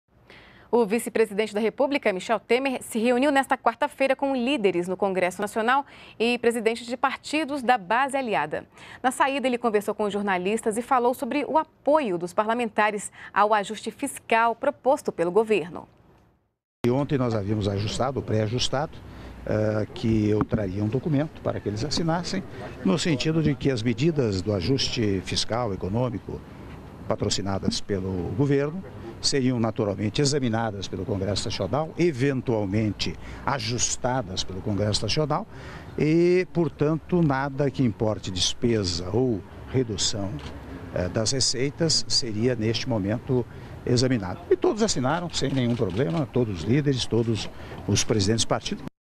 Michel Temer conversa com lideres da base aliada no Congresso Nacional
NBR NOTÍCIAS - 08.04.15: Nesta quarta-feira (8), o vice-presidente da República, Michel Temer, se reuniu com lideres e presidentes de partidos da base aliada no Congresso Nacional. Após o encontro, Temer conversou com jornalistas sobre o apoio dos parlamentares ao ajuste fiscal proposto pelo governo para reduzir gastos e reequilibrar as contas públicas.